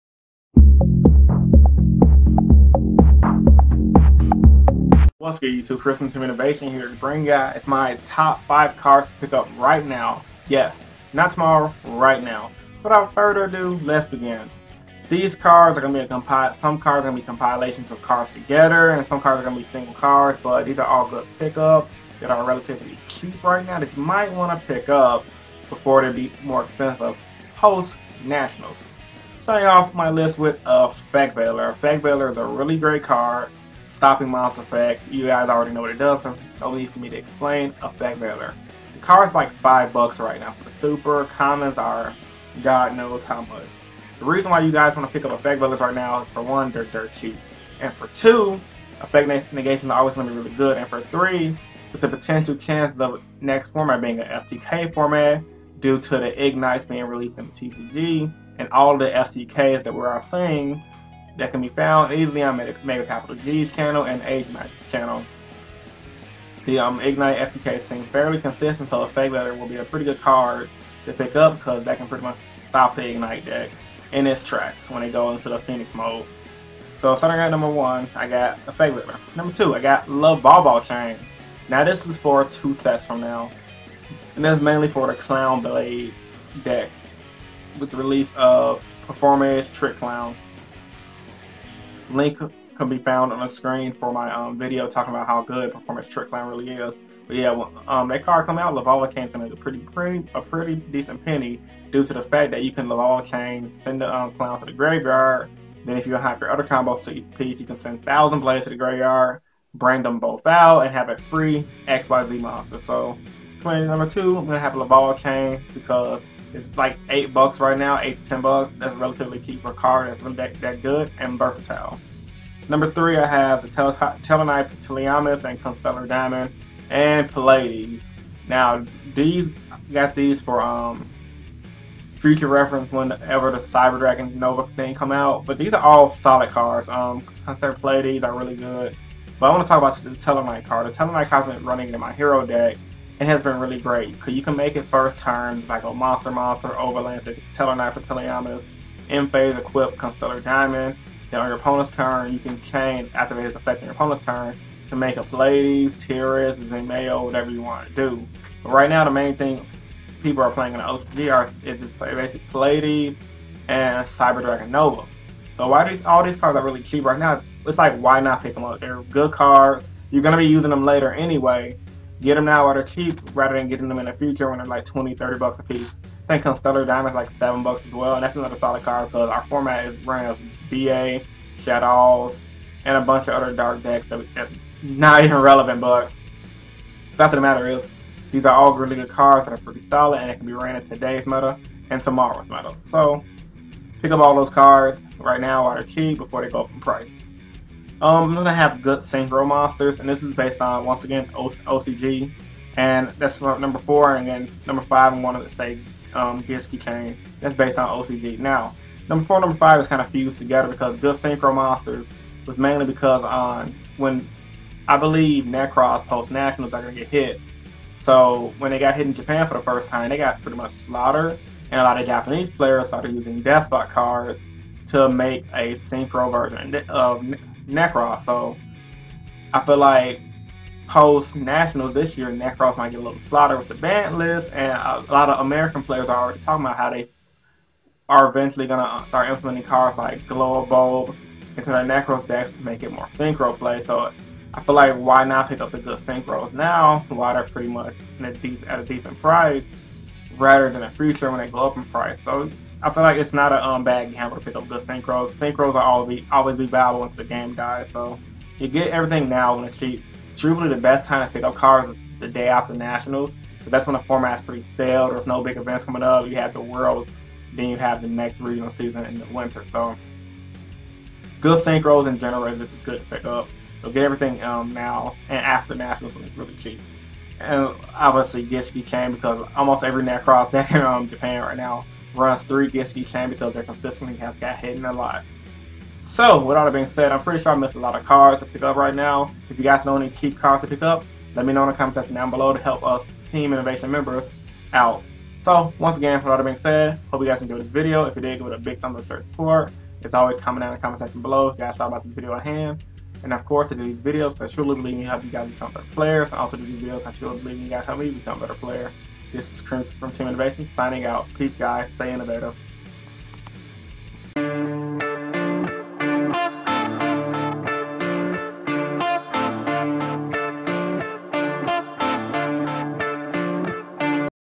Top 5 cards to pick up right now while they are cheap - Yu-Gi-Oh! TCG
Hey guys and welcome back to a yugioh marketwatch video for june 2015 , in this video i will be going over my top 5 cards i think you guys should pick up throughout this month if you want to make some extra cash.